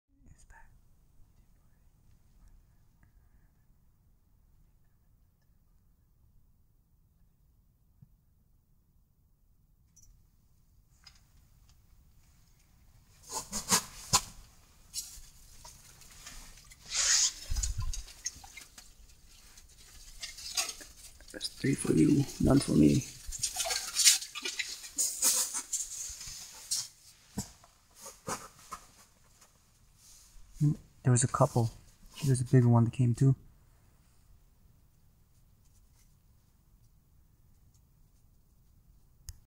White fish fishing 2016. Green bay, WI
Cloudy day but the water was really clear. Me and my dad set up in 3 ft of water and the fish are here. I couldn't get a bite but my dad did in video 2